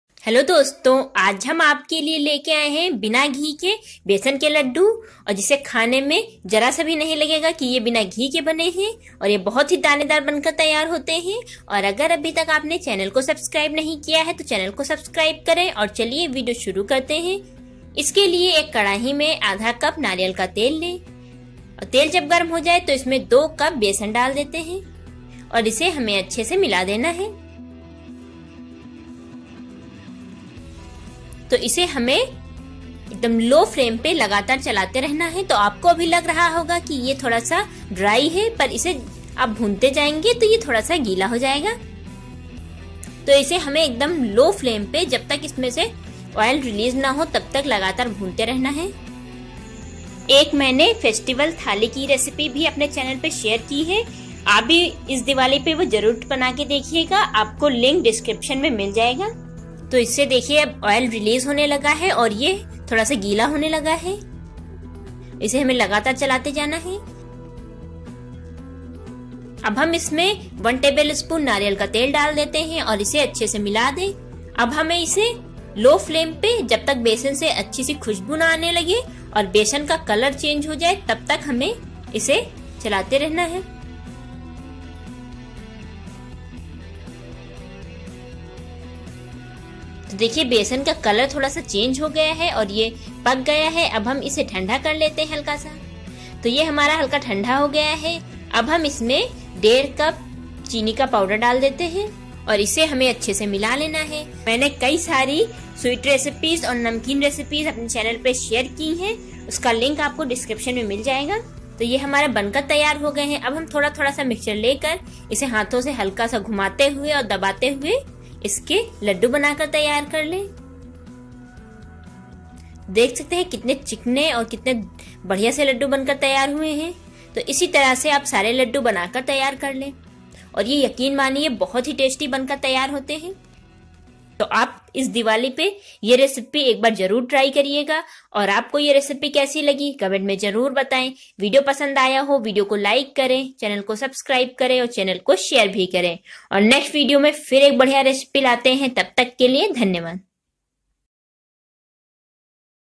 बिना घी के बनाएं बेसन के लड्डू || 𝐛𝐞𝐬𝐚𝐧 𝐤𝐞 𝐥𝐚𝐝𝐝𝐮 || 𝐛𝐞𝐬𝐚𝐧 𝐤𝐞 𝐥𝐚𝐝𝐝𝐮 𝐤𝐚𝐢𝐬𝐞 𝐛𝐚𝐧𝐭𝐚 𝐡𝐚𝐢 || 𝐛𝐞𝐬𝐚𝐧 𝐤𝐞 𝐥𝐚𝐝𝐨𝐨
Ingredients
Gram flour/बेेसन-2 cup
Coconut oil/नारियल तेल-1/2 cup + 1 tbsp